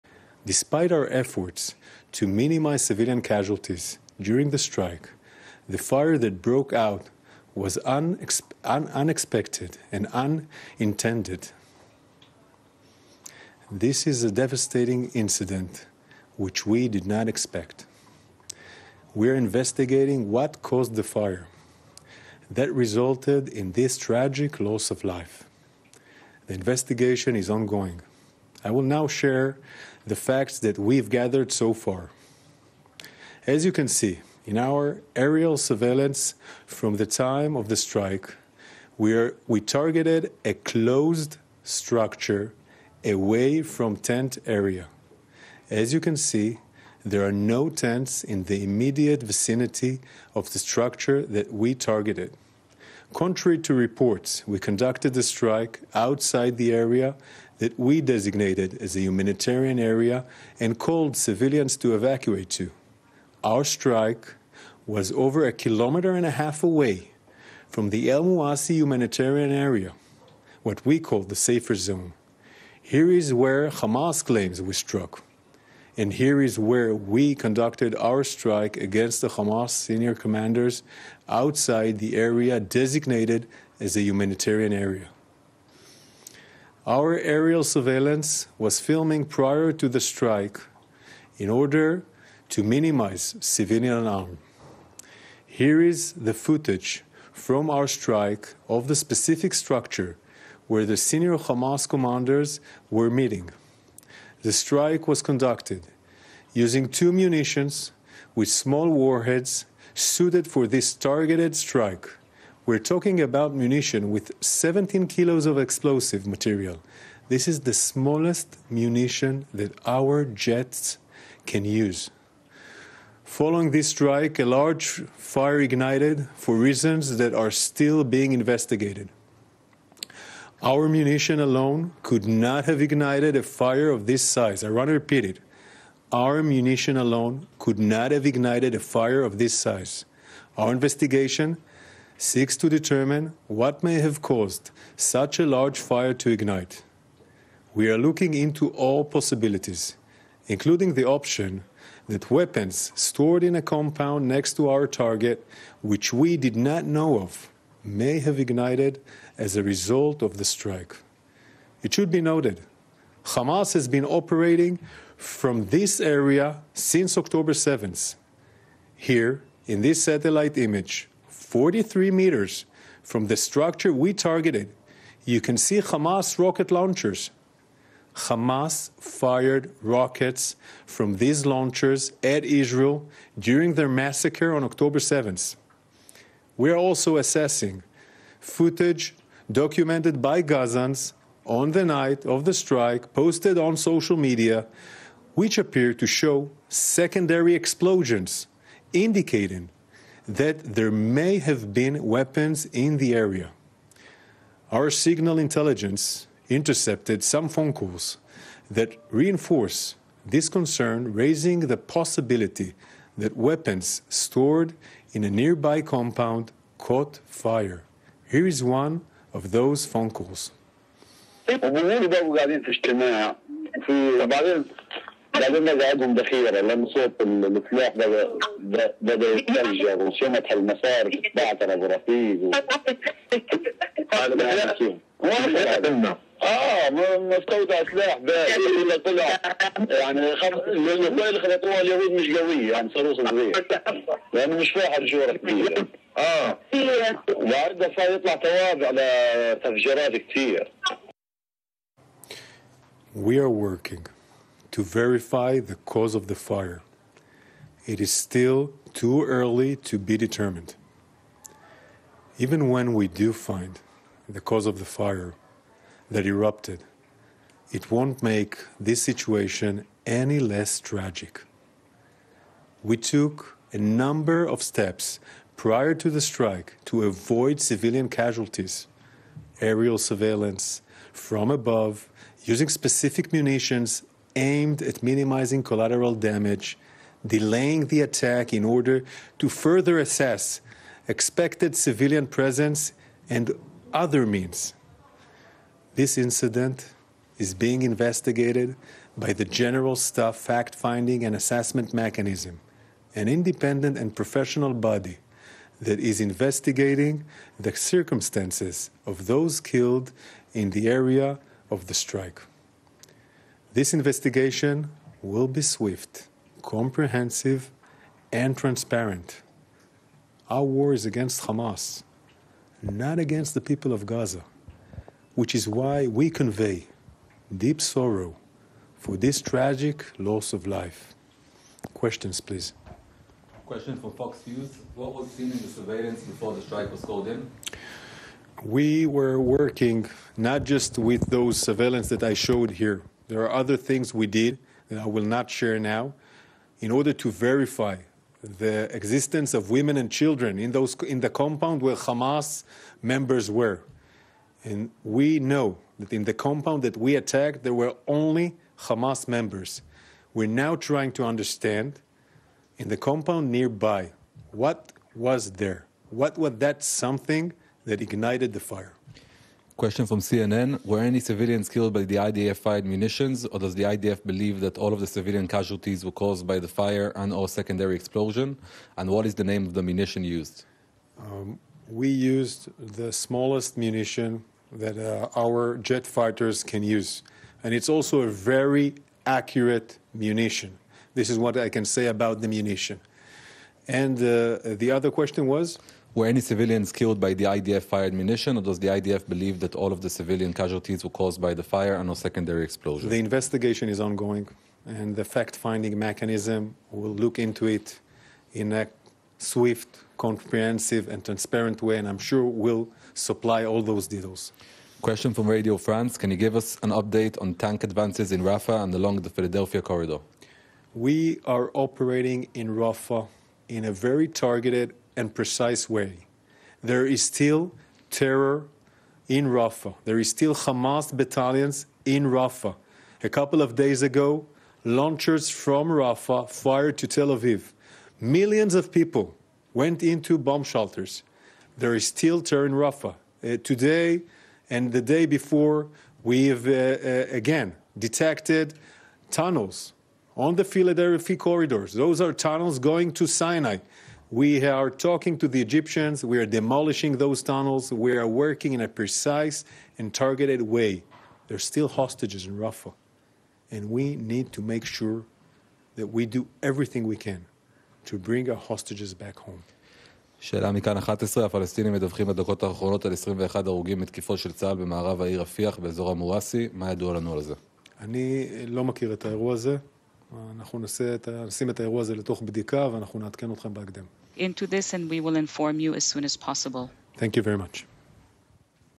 Israel Hamas-War: Fire in Rafah caused by 'secondary explosion' IDF claims
Israel defence forces spokesperson Rear Admiral Daniel Hagari says the fire in Rafah at the weekend was caused by a secondary explosion empathising that 'our ammunition only could not have ignited a fire of this size'.

According to the Hamas-controlled health ministry, at least 45 people were killed in a tent camp after the Israeli airstrike on Sunday, described by Prime Minister Benjamin Netanyahu as a "tragic mistake".